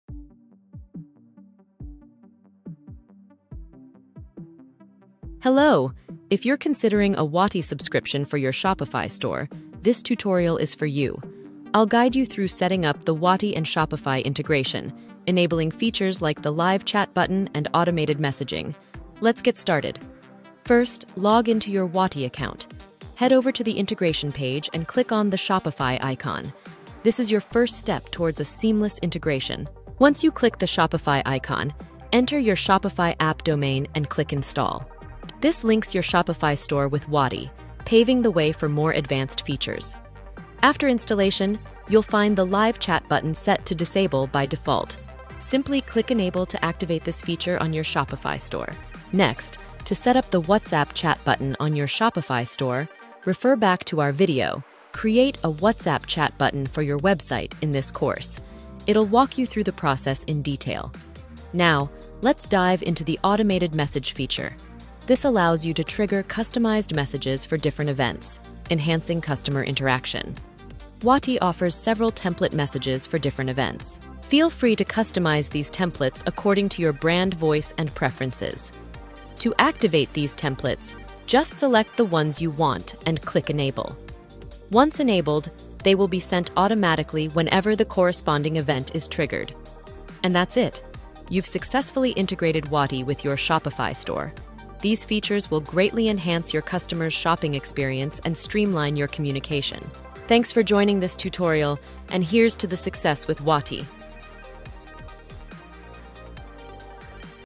Setting up Wati and Shopify Integration
Welcome to our comprehensive guide on integrating Wati with your Shopify store. This tutorial is designed for Shopify store owners looking to enhance their customer service and engagement through Wati. From installing the integration to activating live chat and automated messaging, we cover everything you need to set up Wati seamlessly with your Shopify store.

Tutorial Breakdown:

[0:00 - 0:10] Introduction to Wati for Shopify
[0:11 - 0:18] Getting Started with Integration
[0:19 - 0:25] Logging into Wati & Accessing Integration
[0:26 - 0:39] Linking Shopify with Wati
[0:40 - 0:47] Activating Live Chat Button
[0:48 - 0:56] Setting Up WhatsApp Chat Button
[0:57 - 1:03] Guide to Creating WhatsApp Button
[1:04 - 1:09] Automating Messages
[1:10 - 1:23] Customizing Template Messages
[1:24 - 1:29] Enabling Automatic Messages
[1:30 - 1:39] Integration Complete & Benefits

Integrating Wati with your Shopify store unlocks powerful tools to connect with your customers directly through WhatsApp. This tutorial ensures a smooth setup process, allowing you to leverage live chat functionalities and automated messaging to improve your store's customer interaction and satisfaction.

Start transforming your customer service today with Wati and Shopify.